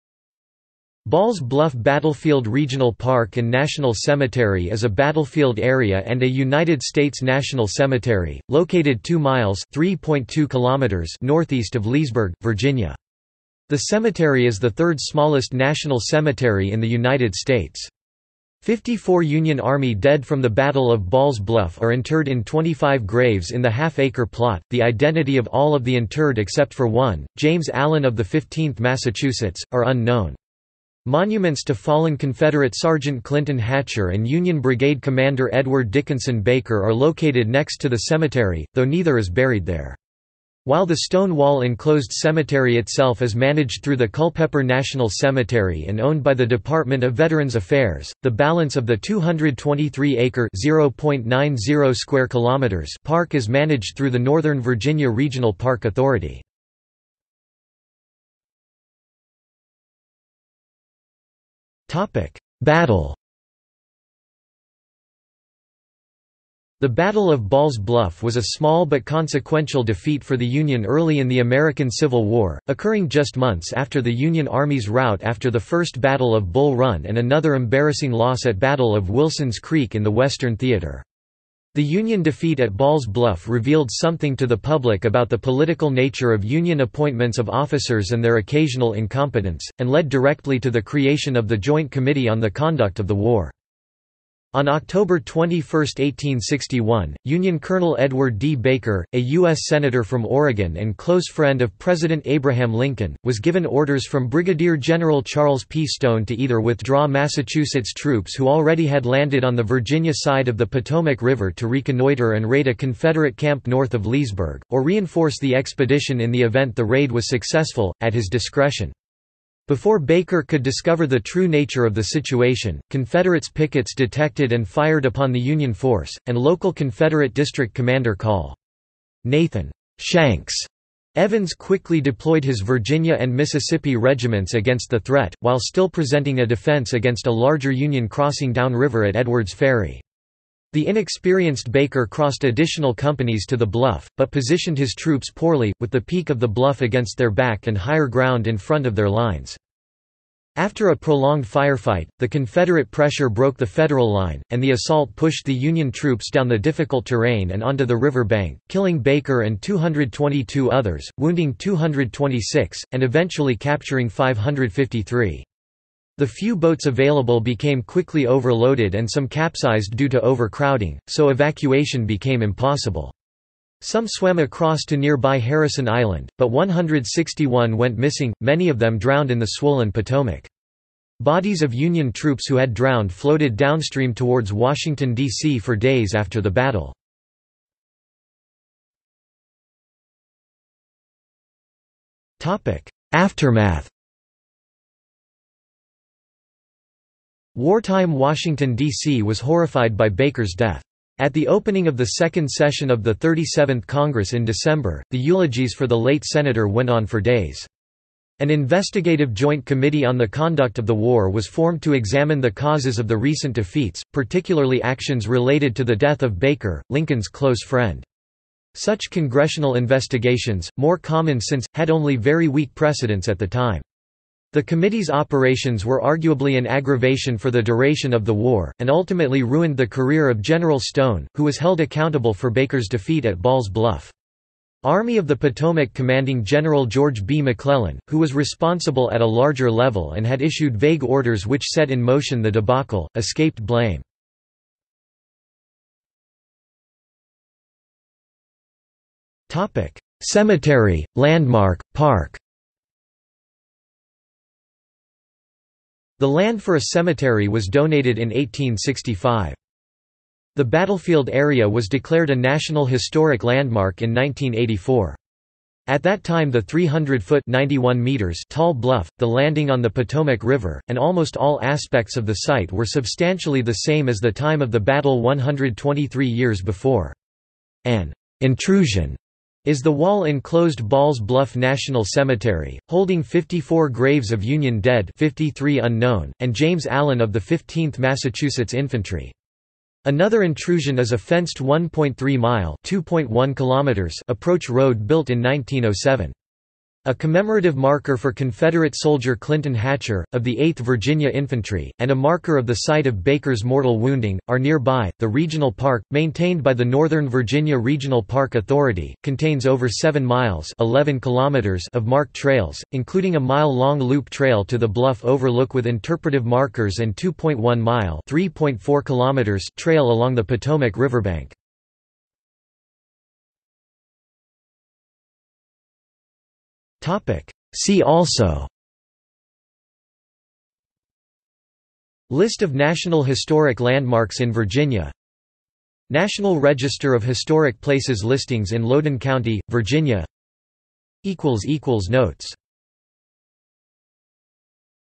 Ball's Bluff Battlefield and National Cemetery | Wikipedia audio article
This is an audio version of the Wikipedia Article:
Ball's Bluff Battlefield and National Cemetery

00:00:58 1 Battle
00:03:08 2 Aftermath
00:04:05 3 Cemetery, landmark, park
00:05:23 4 See also

- Socrates

SUMMARY
Ball's Bluff Battlefield Regional Park and National Cemetery is a battlefield area and a United States National Cemetery, located 2 miles (3.2 km) northeast of Leesburg, Virginia.  The cemetery is the third smallest national cemetery in the United States. Fifty-four Union Army dead from the Battle of Ball's Bluff are interred in 25 graves in the half-acre plot; the identity of all of the interred except for one, James Allen of the 15th Massachusetts, are unknown. Monuments to fallen Confederate Sergeant Clinton Hatcher and Union brigade commander Edward Dickinson Baker are located next to the cemetery, though neither is buried there. While the stone wall-enclosed cemetery itself is managed through the Culpeper National Cemetery and owned by the Department of Veterans Affairs, the balance of the 223-acre (0.90 km2) park is managed through the Northern Virginia Regional Park Authority.